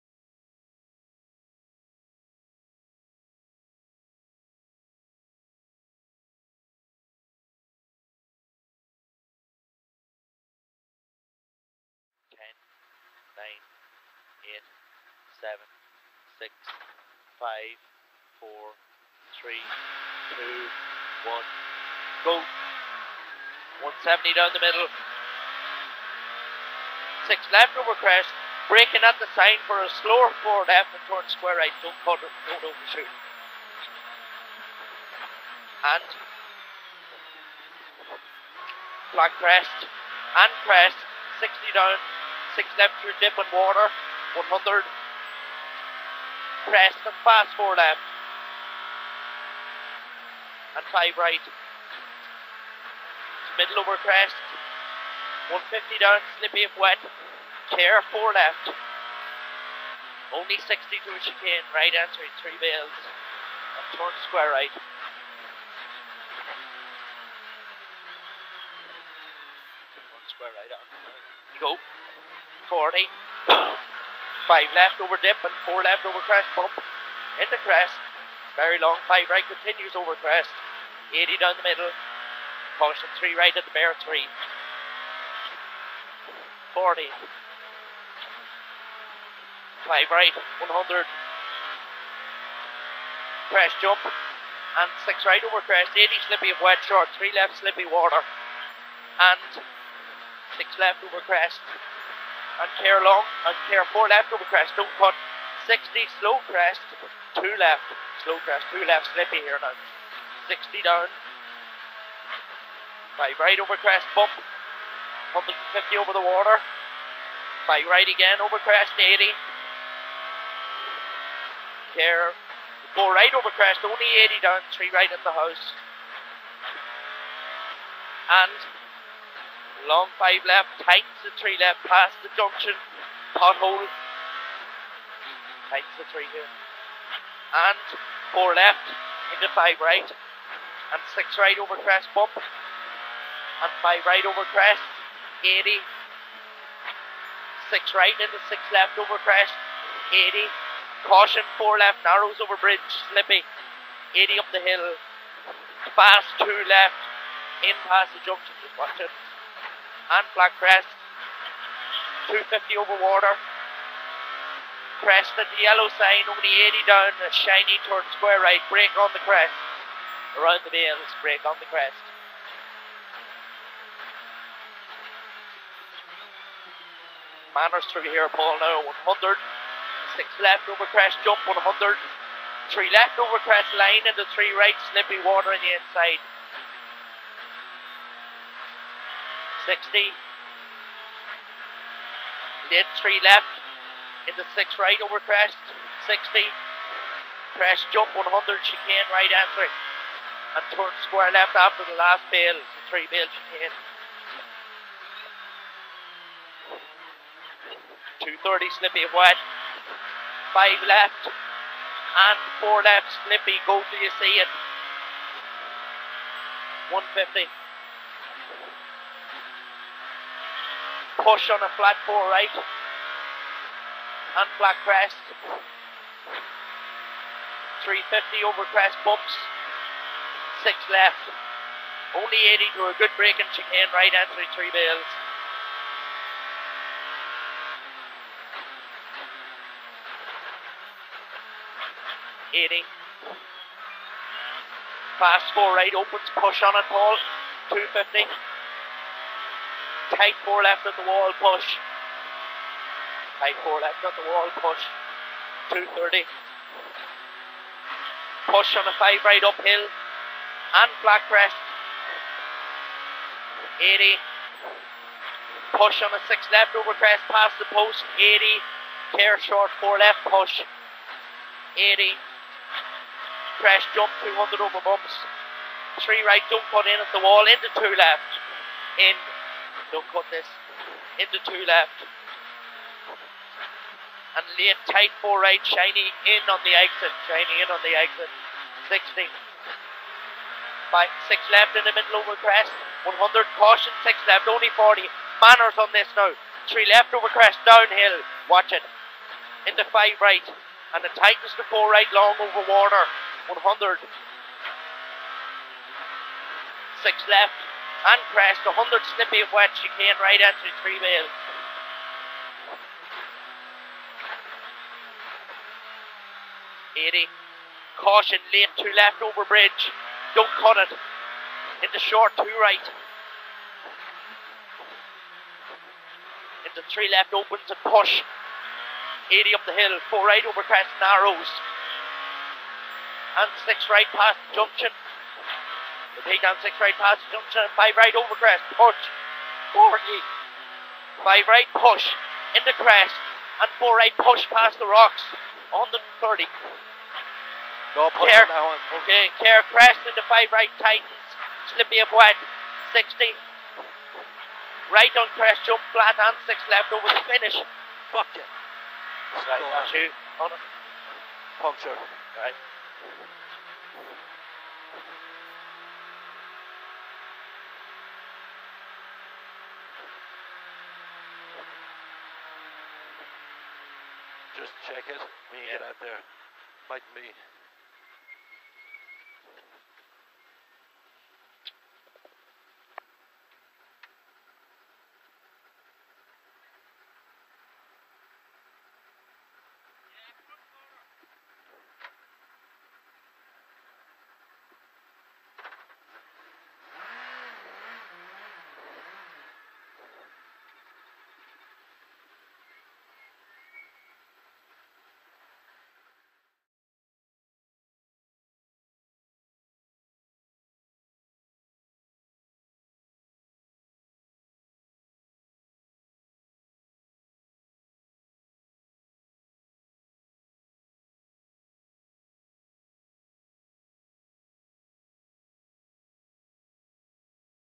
2016 Galway International Rally - Paul Rowley & Patrick Brides - Stage 11
SVS Incar Camera Footage of Paul Rowley & Patrick Brides in their Skoda Fabia S2000.
Upload Requested By: Patrick Brides.

This is Stage 11 of the 2016 Corrib Oil Galway International Rally.